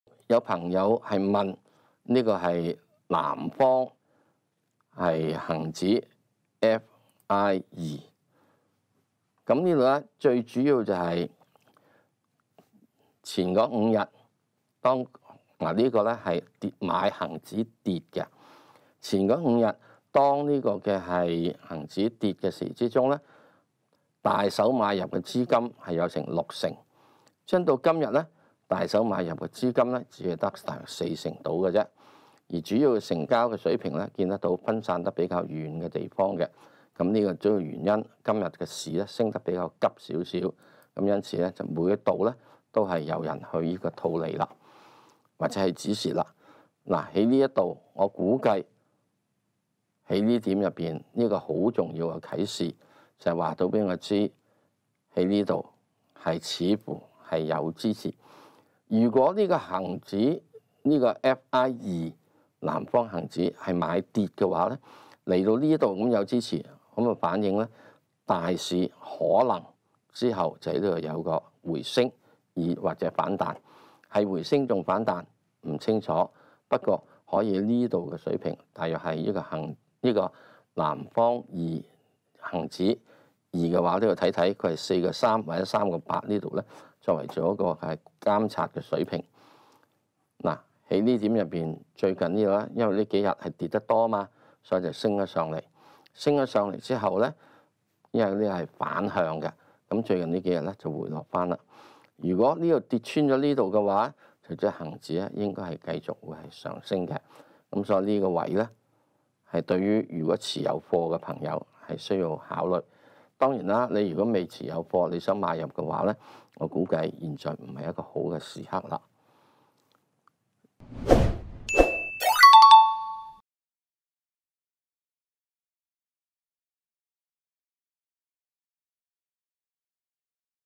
【石Sir午市閒談】（精華）港股重上二萬八FI二南方恒指向下 買賣策略如何？
FI二南方恒指
港股反彈，大市重上28000點，反向ETF FI二南方恒指向下，買賣策略如何？